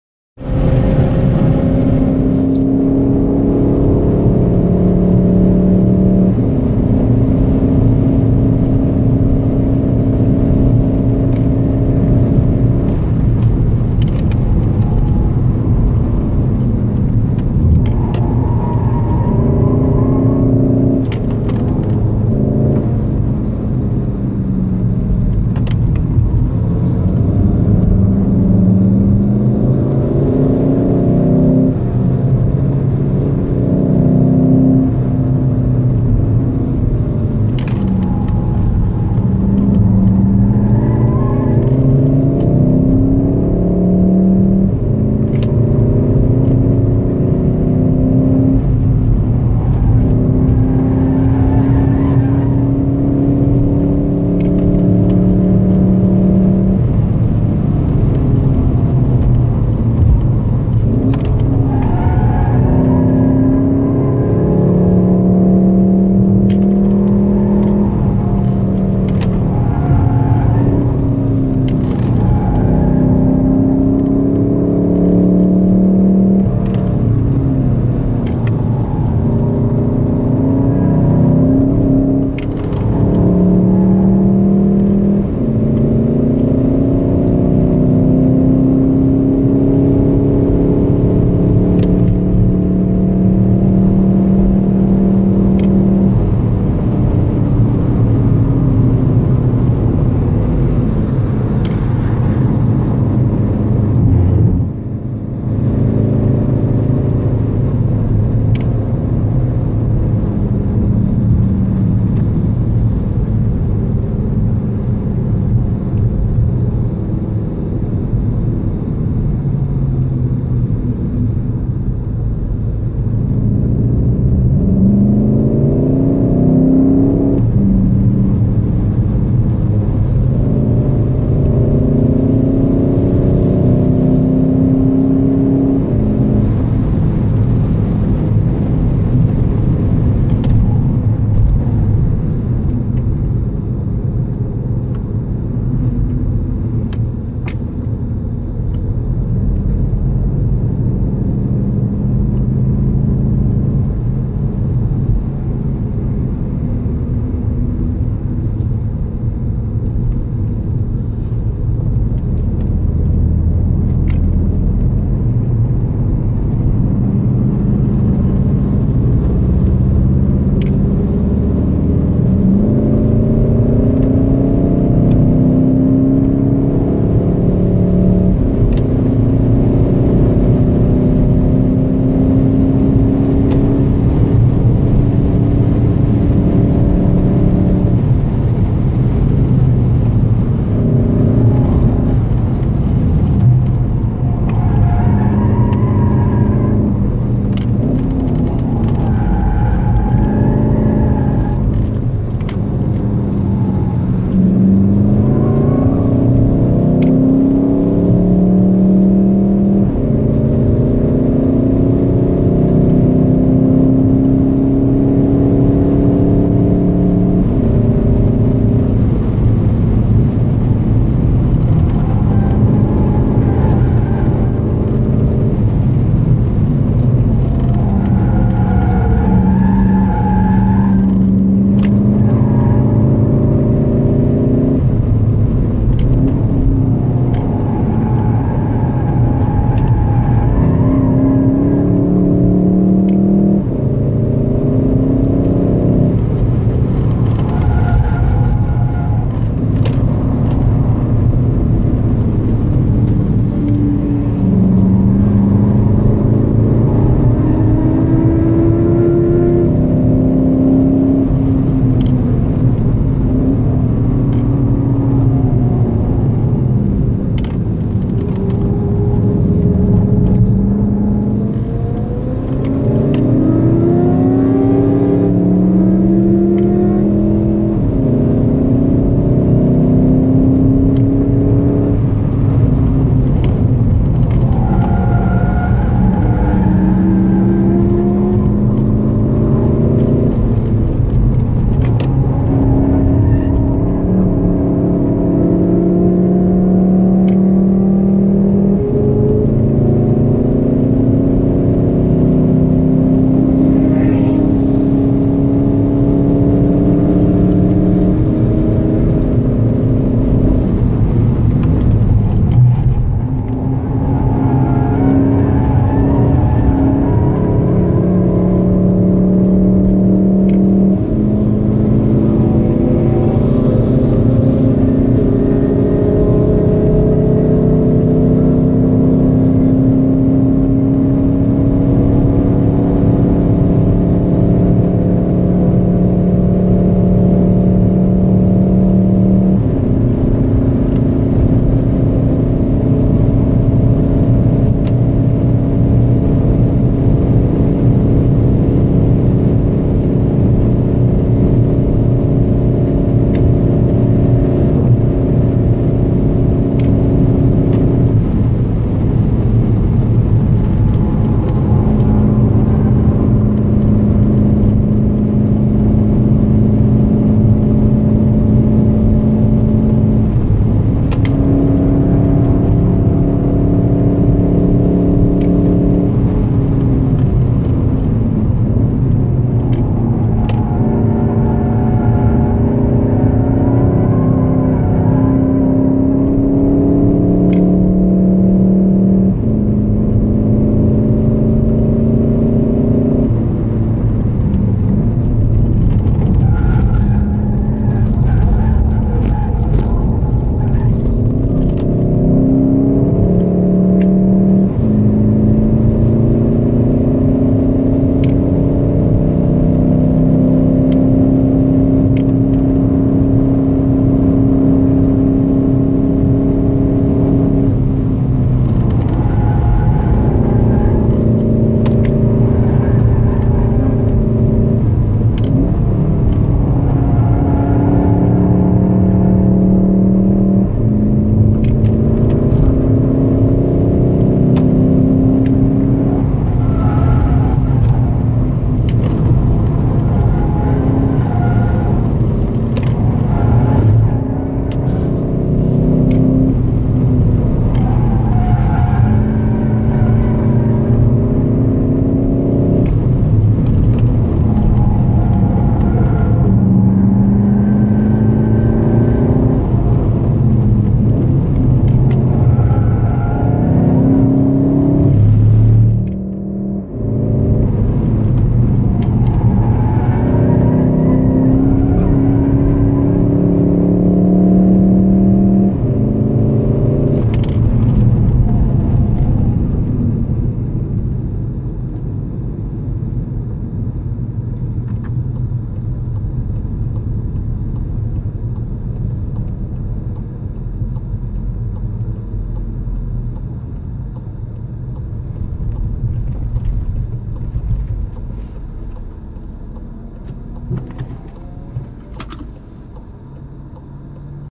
𝗦𝘁𝗼𝗰𝗸 𝗚𝗼𝗹𝗳 𝟳 𝗚𝗧𝗗 & 𝗠𝟯 𝗘𝟰𝟲 𝗡𝗼𝗿𝗱𝘀𝗰𝗵𝗹𝗲𝗶𝗳𝗲 [𝗕𝘁𝗚] 𝟬𝟵.𝟬𝟵.𝟭𝟲 𝗖𝗵𝗮𝗼𝘀 𝗟𝗮𝗽 | 𝗕𝗶𝗴 𝗖𝗿𝗮𝘀𝗵 𝗠𝗲𝗴𝗮𝗻𝗲 | 𝗖𝗿𝗮𝘀𝗵 𝗠𝗼𝘁𝗼𝗿𝗯𝗶𝗸𝗲
👇👇👇 Car Specs, Links & more Information 👇👇👇
⏩⏩⏩ Storyboard / Time Stamps:
0:00 Start | The Bilstein Bridge
0:15 Into the Sunset...
0:27 Ferrari F 488
0:35 Porsche 996 Carrera
1:40 Intervention Car w/ blinking Lights = No Overtaking!
2:20 Big Crash Schwedenkreuz | Megane RS
4:10 Brave Biker | Hats off!
5:00 GT3
5:20 Massive Diesel Power ;-))
6:30 Carraciola Carousel
7:35 Brünnchen | Youtube Corner
8:00 Crash Motobikes

⏩⏩⏩ Equipment:
OBD II Bluetooth Dongle
10 Hz external GPS Device

⏩⏩⏩ Car Specs:
2015 Golf MK 7 GTD | all bone stock | komplett original
Samsung S5 & Harry's Laptimer - Reifen/Tires: Bridgestone S001 in 225/40/18

Touristic Driving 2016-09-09 | 9.9.2016 | 9.9.16 | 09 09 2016 | 09 09 16